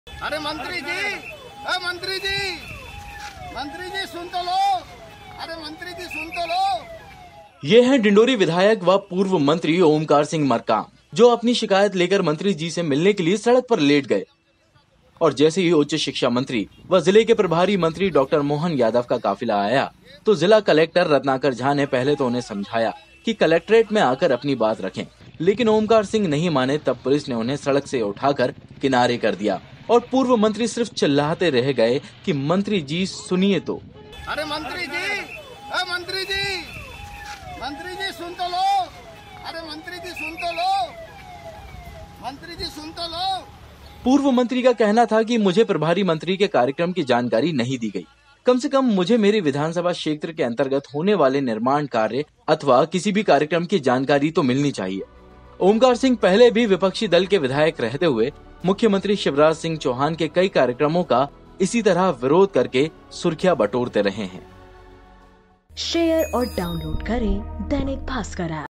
पुलिस ने विधायक को टांगाटोली कर उठाया
पुलिस ने विधायक को टांगाटोली कर उठाया: डिंडौरी के प्रभारी मंत्री मोहन यादव से मिलने से रोका तो सड़क पर बैठ गए MLA ओमकार सिंह, पुलिस हाथ-पैर पकड़कर साइड में ले गई